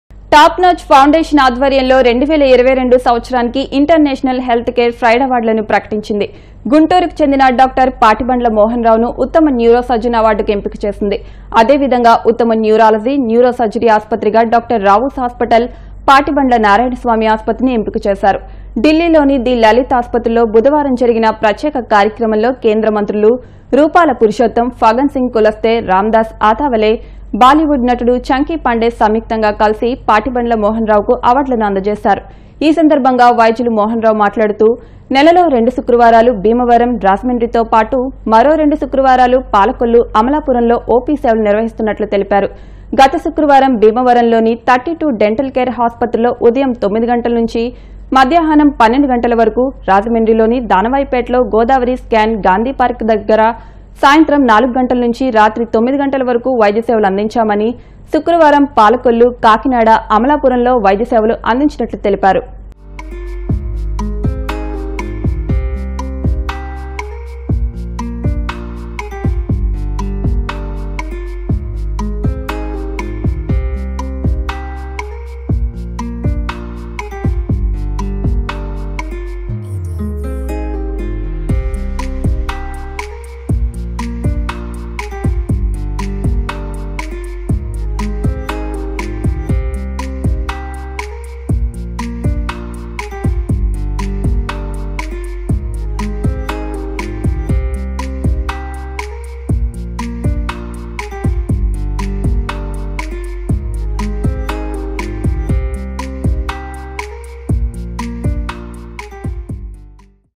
Best Neurology and Neurosurgery award 2022 - Dr. Rao's Neuro Hospital
Dr. Mohana Rao Patibandla was awarded the best neurosurgeon of the year 2022 by Union cabinet ministers and, Dr. Rao’s hospital, Guntur was awarded as the best neurology and neurosurgery hospital of the year at Newdelhi, the Lalit hotel. Dr. Mohana Rao Patibandla is visiting Bhimavaram, Palakollu, Rajahmundry, Amalapuram,  Kakinada, Chilakaluripet, Narsaraopet, Piduguralla, Ongole, Nellore twice a month to serve neuro patients.